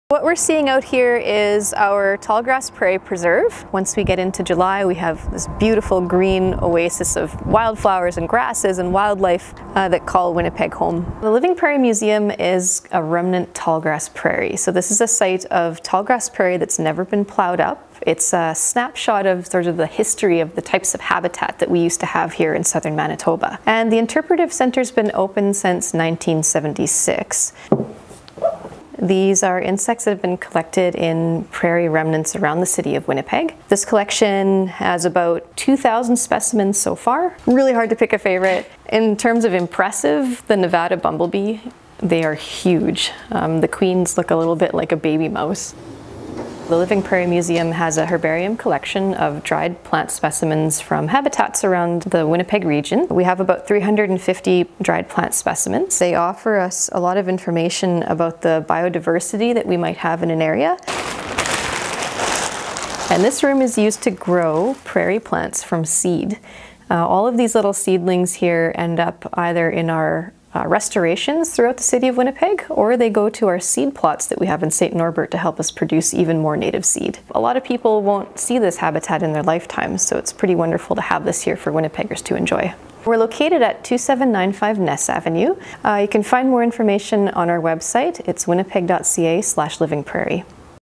A look at the Living Prairie Museum
The Living Prairie Museum features 12 hectare tall grass prairie preserve located inside the city of Winnipeg. It offers a chance to get up close to prairie wildlife.